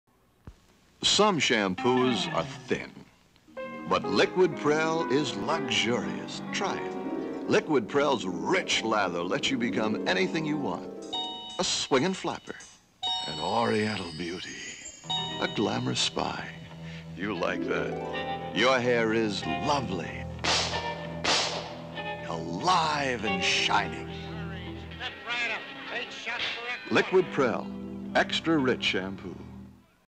VINTAGE COMMERCIAL - PRELL SHAMPOO
00:00 (TC: 01.40.46): Prell Shampoo Commercial. Woman shampooing her hair., being silly with shampoo in her hair.